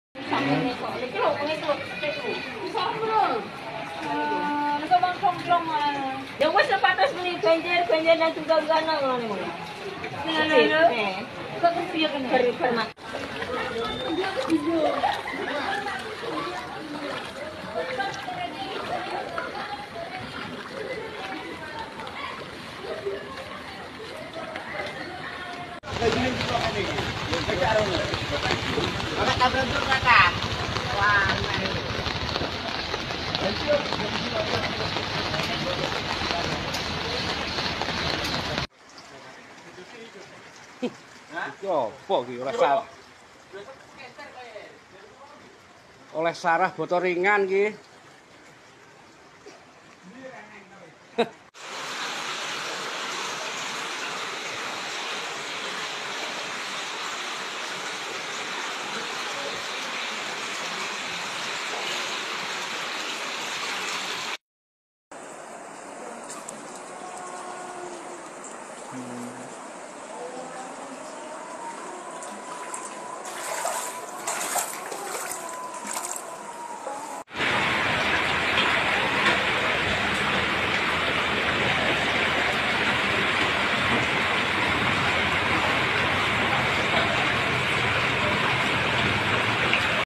Banjir Bandang Terjang Dander Bojonegoro
Banjir Bandang Minggu 18/2/2024